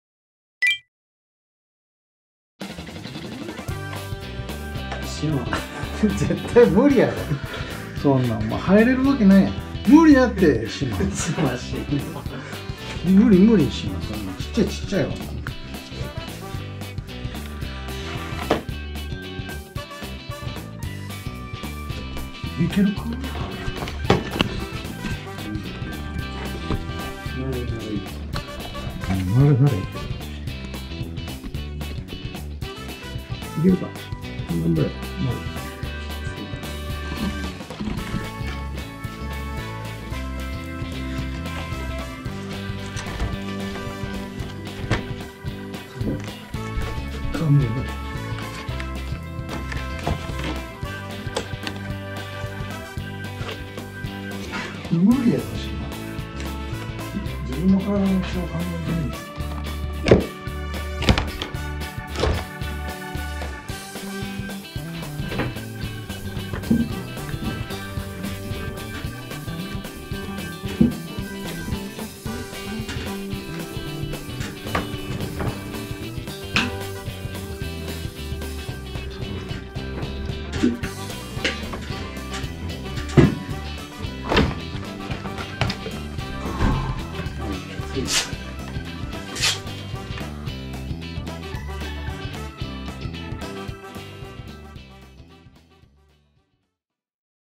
【まるしま】ティッシュの箱【入れるかな？】
猫ってすぐに大きくなりますねー。

急激に大きくなる体の変化についていけるのでしょうか？